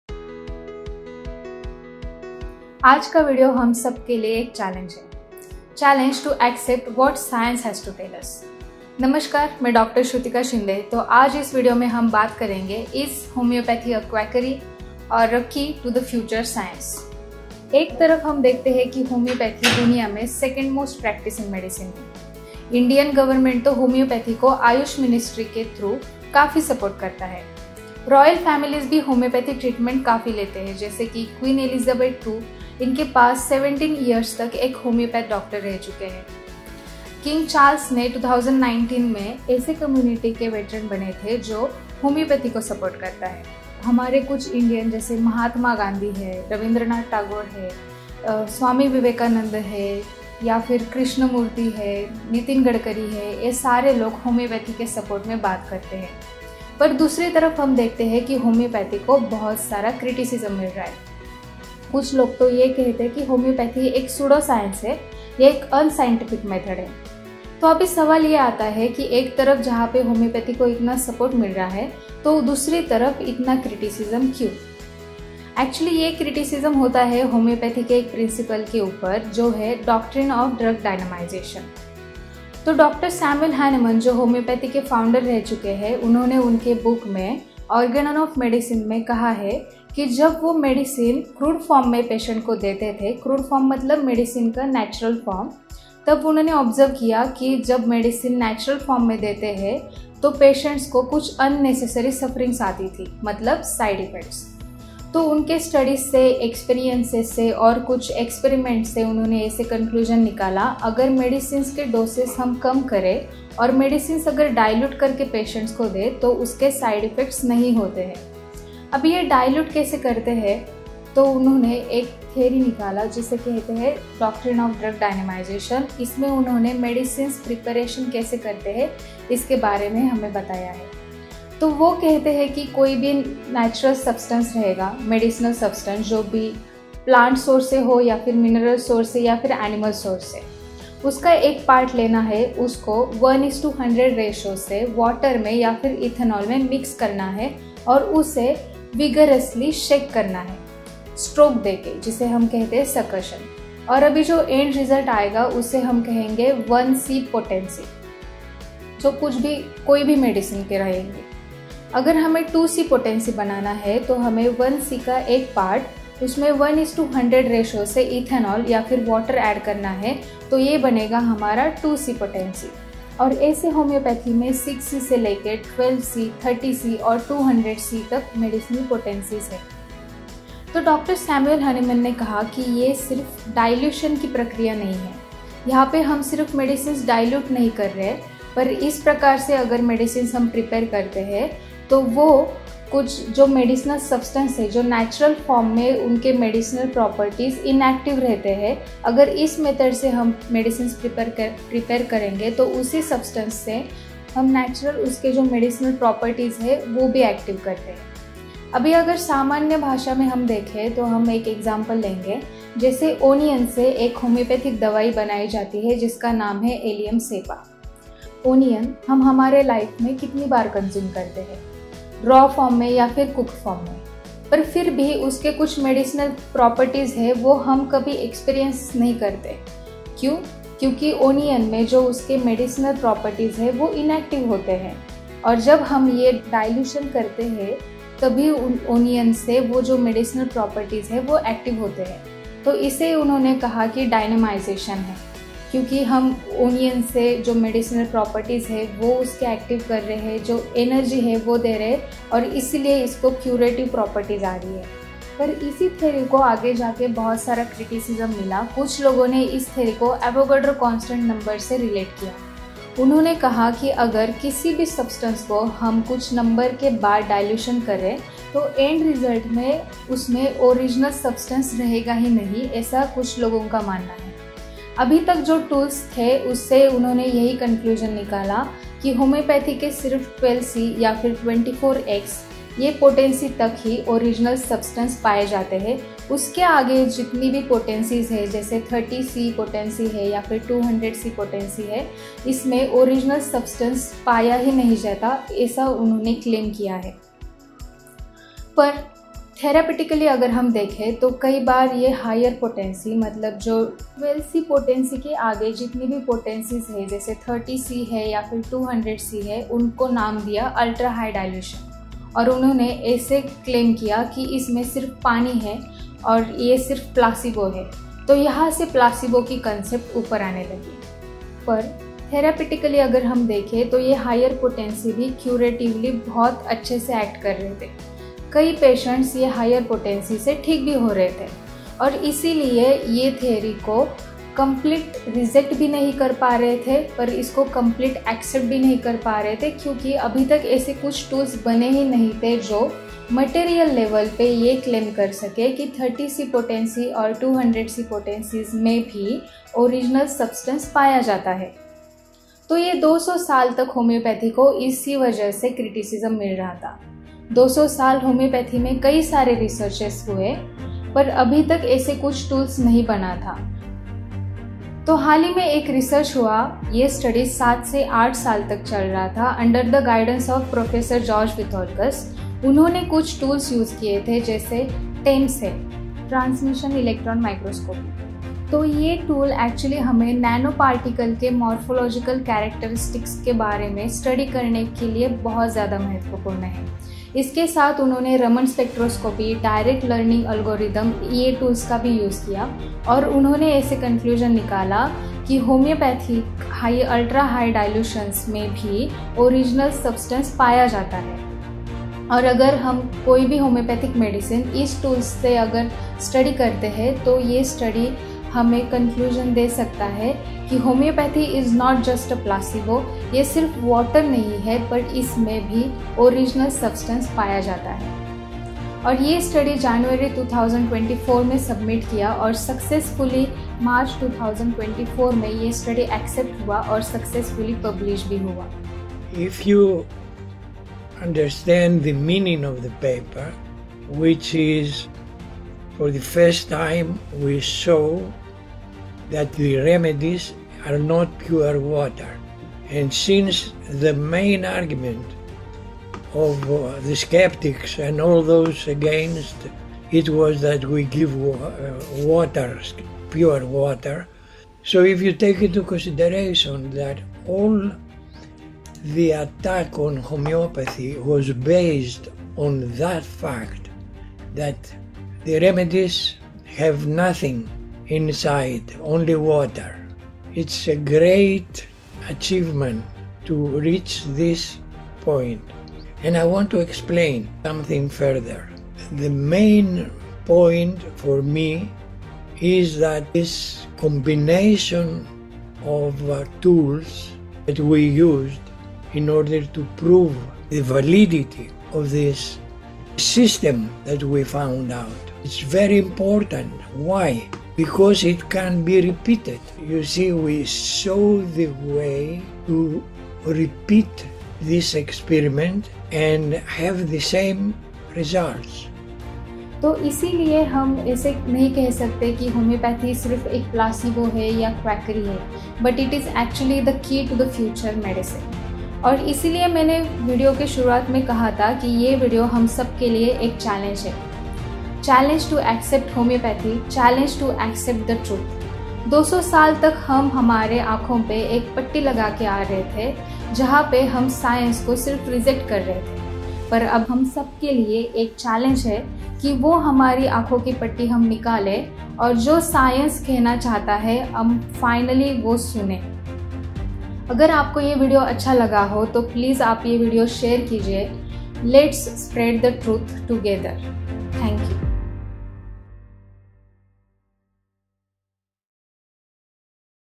The most significant research in history of medicine | George Vithoulkas | Dr Shrutika Homeopathy
The study that the homeopathic community was waiting for the last 200 years

Dear Homeopaths and Friends of Homeopathy,

For the past eight years, the dedicated Research Team of International Academy of Classical Homeopathy, under the guidance of Professor George Vithoulkas and the thorough work of the Head of the Team, Dr Camelia Grosan, has been diligently working to provide conclusive evidence refuting the long-standing skepticism surrounding homeopathic remedies.

Through a meticulous combination of advanced analytical techniques including Transmission Electron Microscopy (TEM), Raman Spectroscopy, and Deep Learning (DL) algorithms, this study successfully confirms that homeopathic remedies are active. The investigation focused on evaluating the morphological characteristics of highly diluted solutions of gold (Aurum Met) prepared in both ethanol and water bases across three different levels of potentization (6C, 30C, and 200C). The findings presented in this research definitively settle the debate on the   high potencies in homeopathy.

The analytical method employed in this study, we believe, will open new vistas in medicine.

 Dr. Shrutika